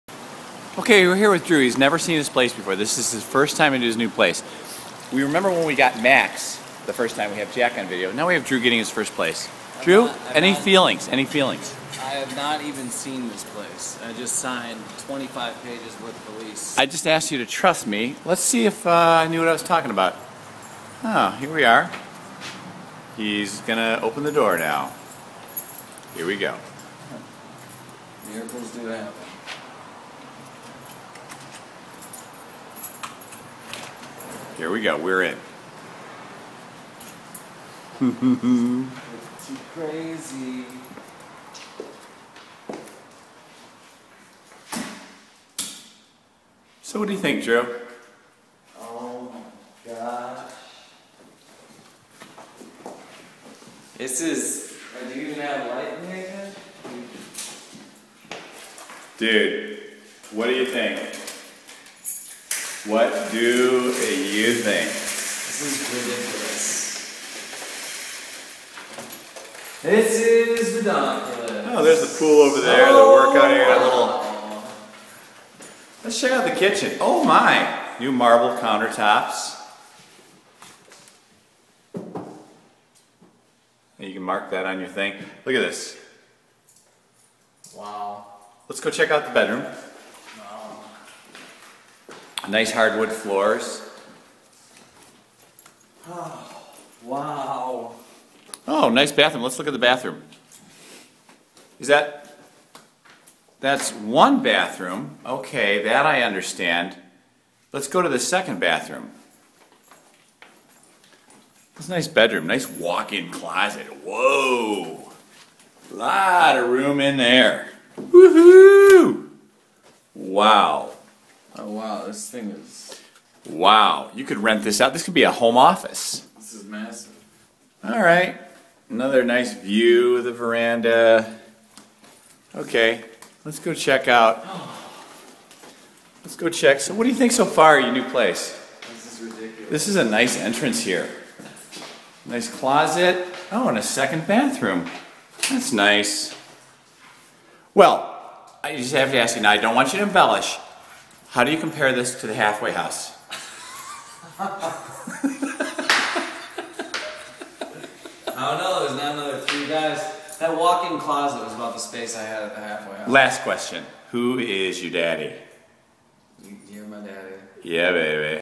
Drew Swan First Apartment
Drew and Dad nail the perfect pad after a frantic 3 day search.